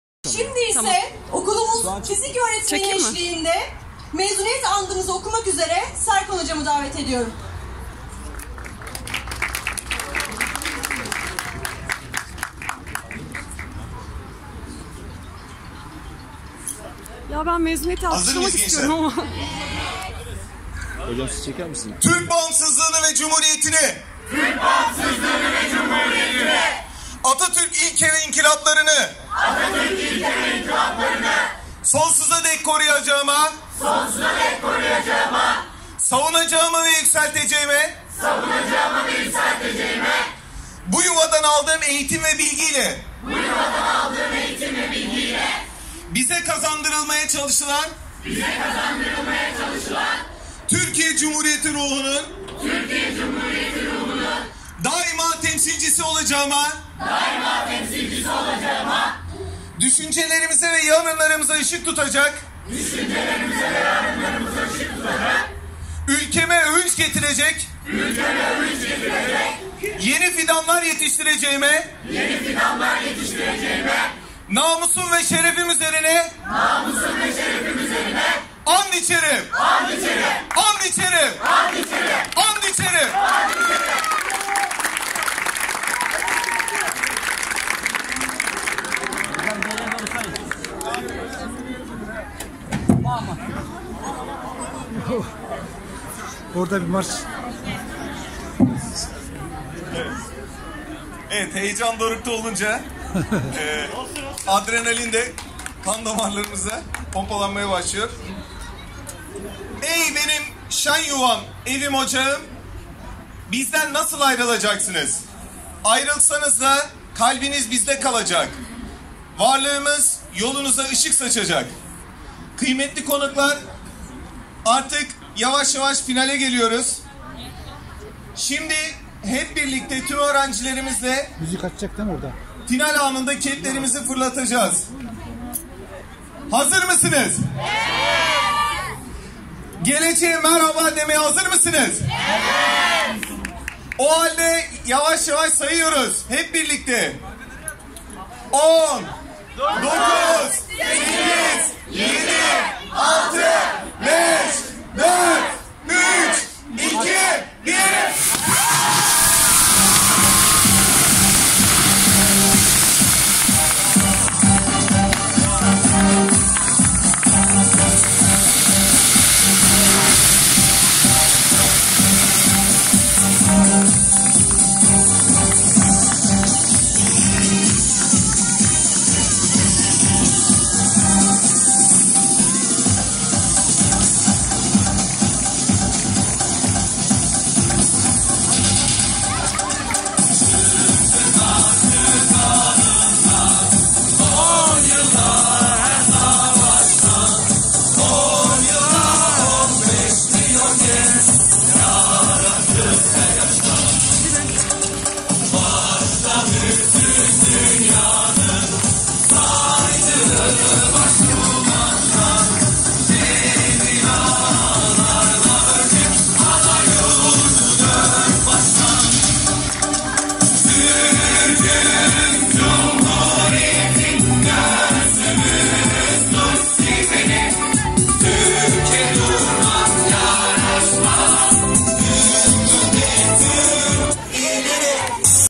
Mezuniyet Andı.... (Final Okulları)
Mezuniyet töreni için mezuniyet andı...